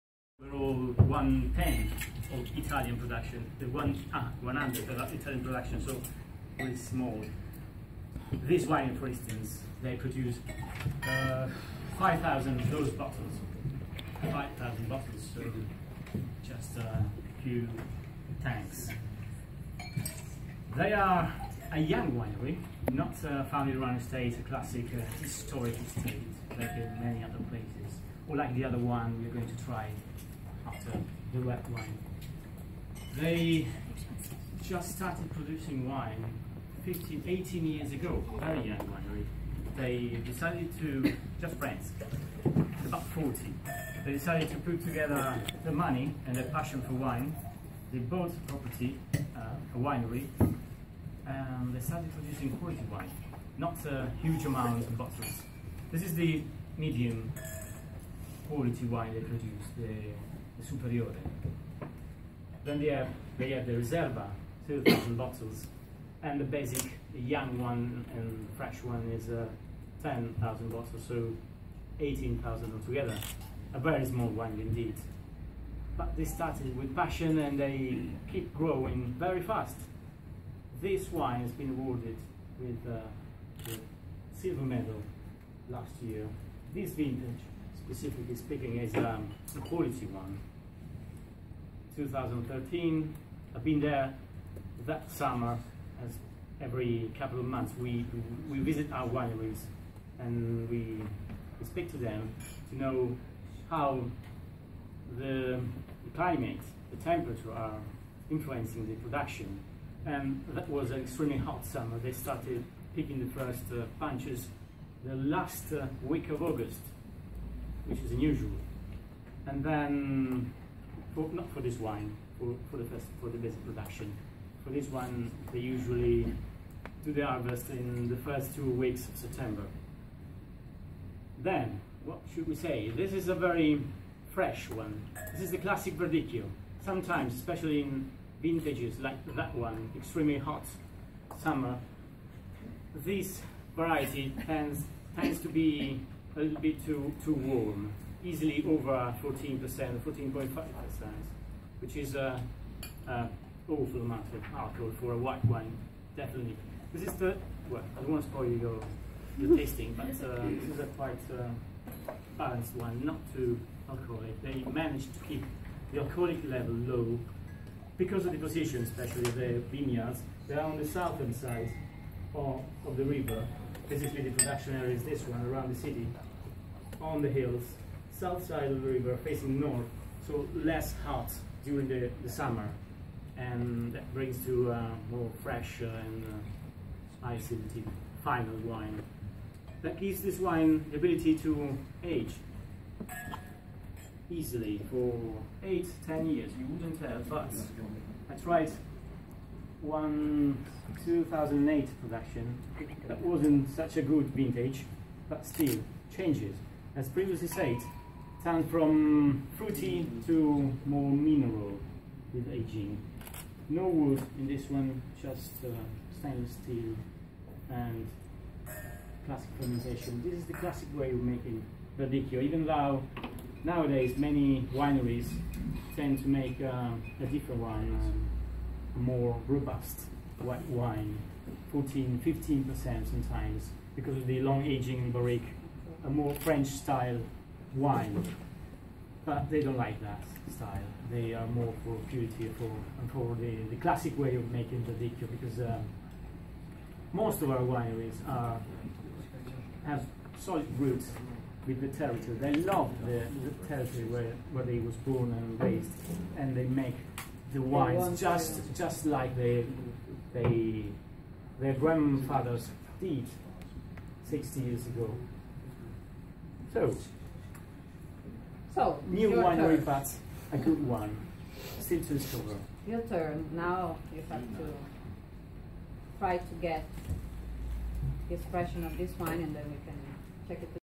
UK Sommelier Association Lesson: Verdicchio Ripa Marchetti
Our Manager Roberto is been invited by the UK Sommelier Association to give a lesson about Le Marche wine